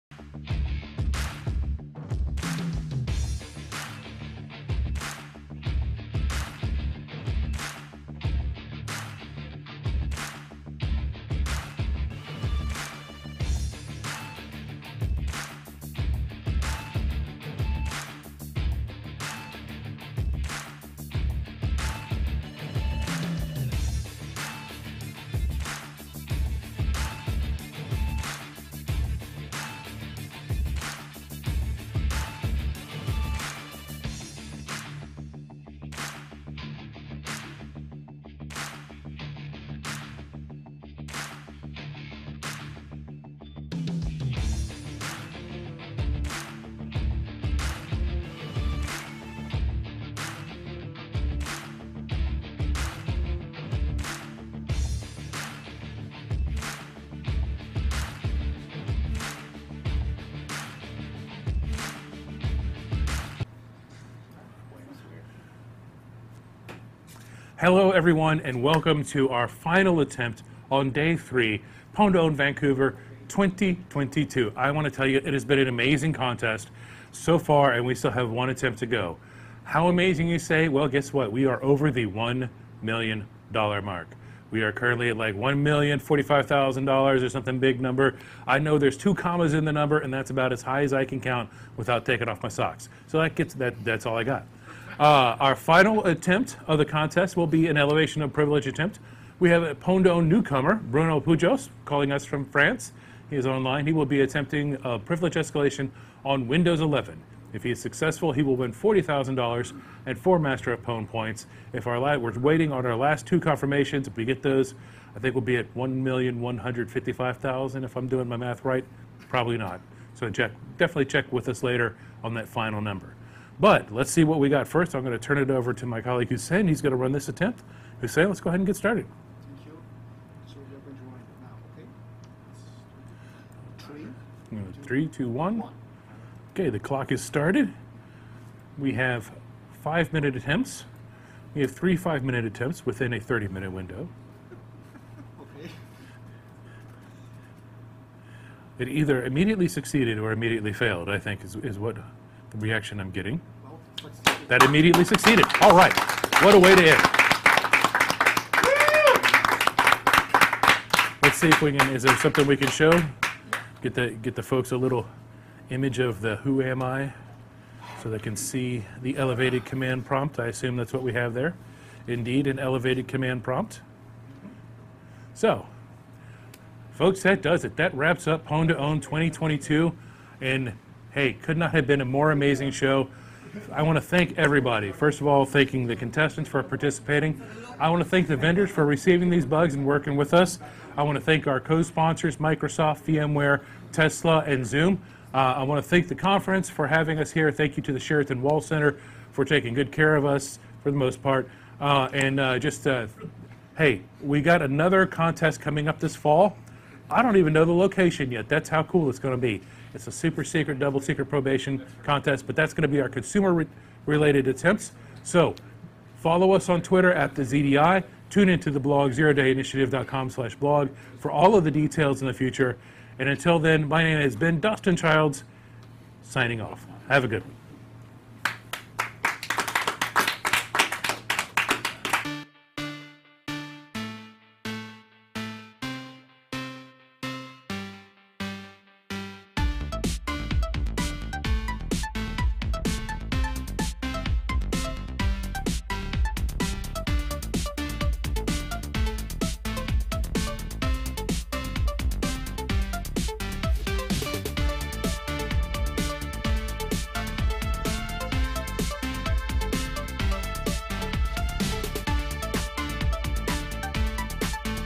Pwn2Own Vancouver 2022 - Bruno PUJOS vs Microsoft Windows 11
Our final attempt of Pwn2Own Vancouver 2022 has Bruno PUJOS (@brunopujos) from REverse Tactics targeting Microsoft Windows 11 in the Local Elevation of Privilege category. If he succeeds, he will earn $40,000 and 4 points towards Master of Pwn.